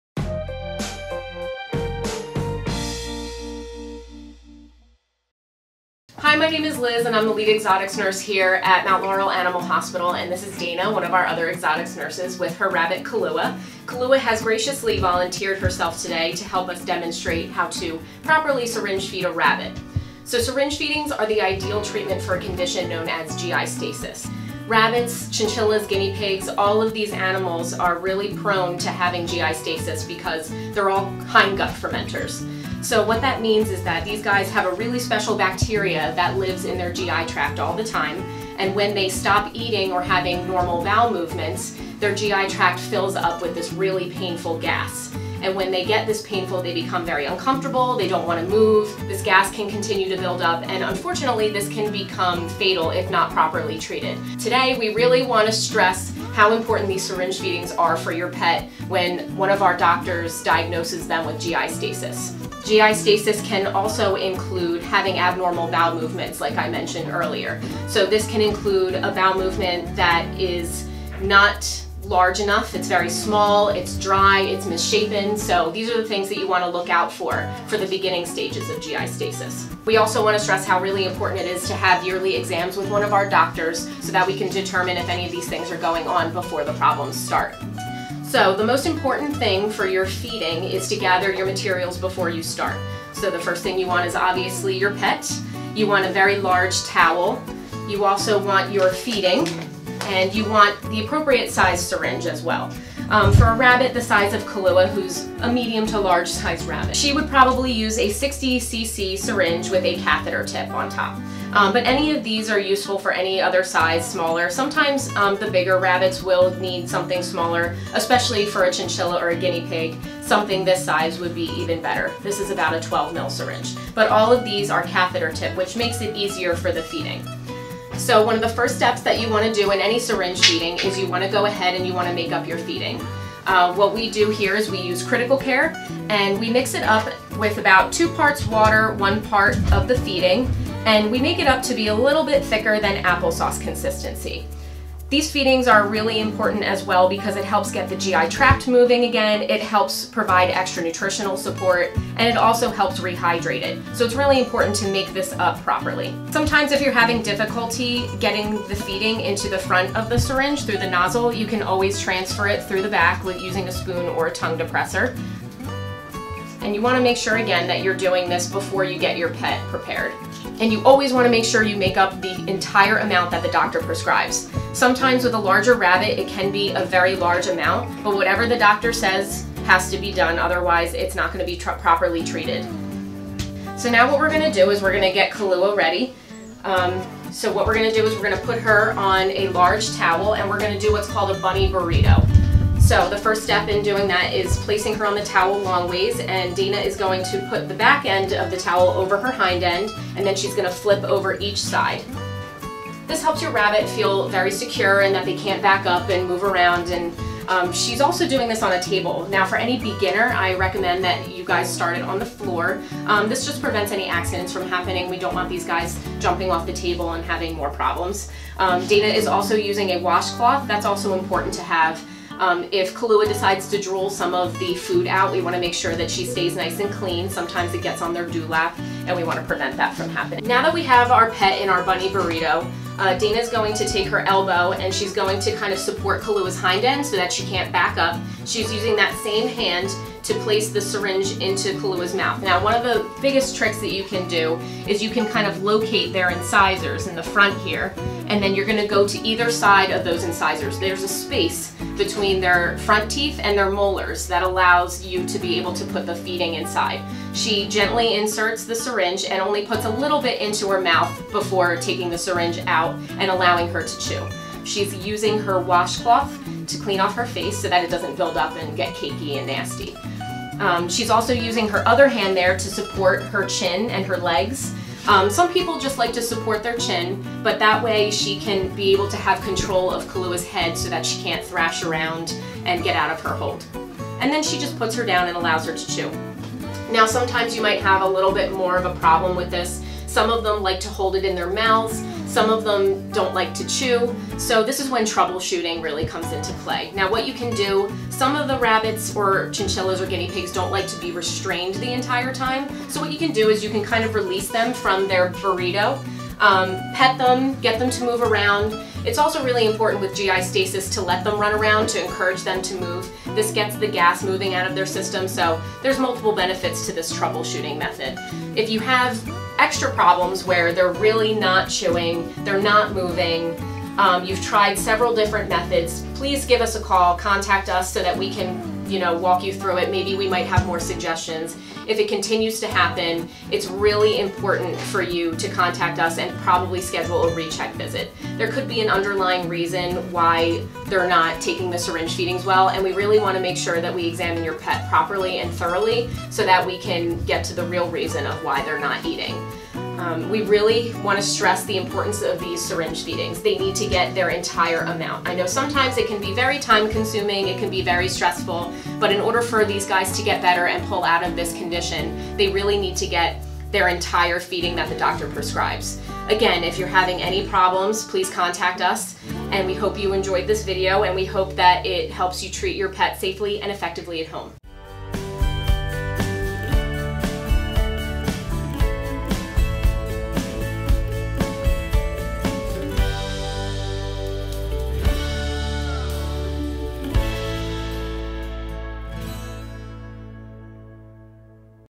Syringe Feeding Rabbits and Other Small Animals
In this video Certified Veterinary Technicians Liz and Dana show the proper way to syringe feed a rabbit, with the help of Dana's rabbit, Kahlua.